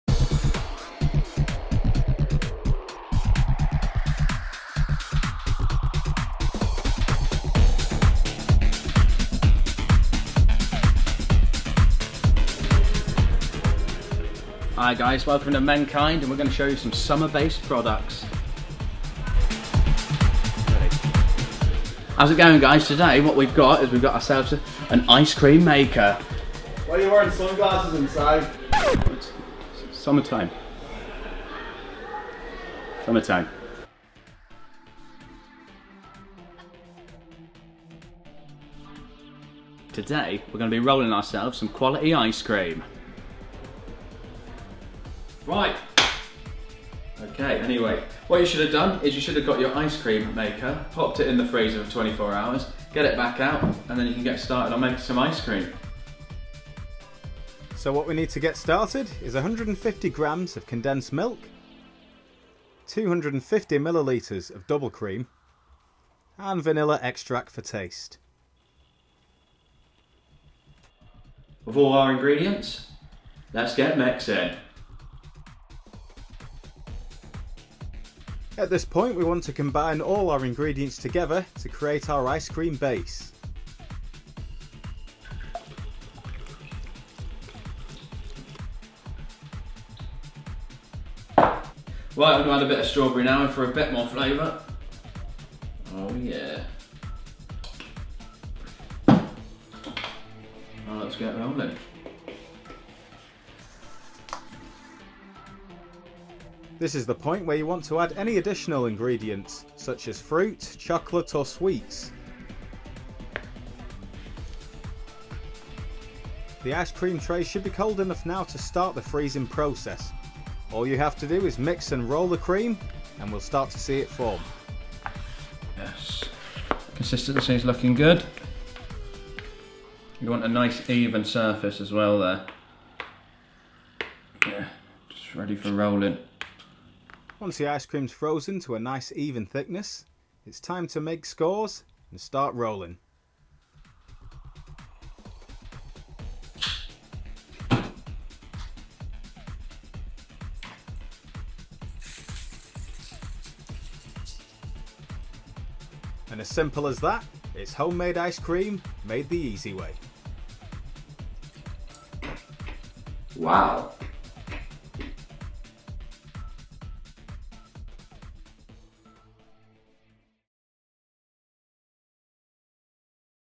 Rolled Ice Cream Maker
Make delicious rolled ice cream at home. It's easy!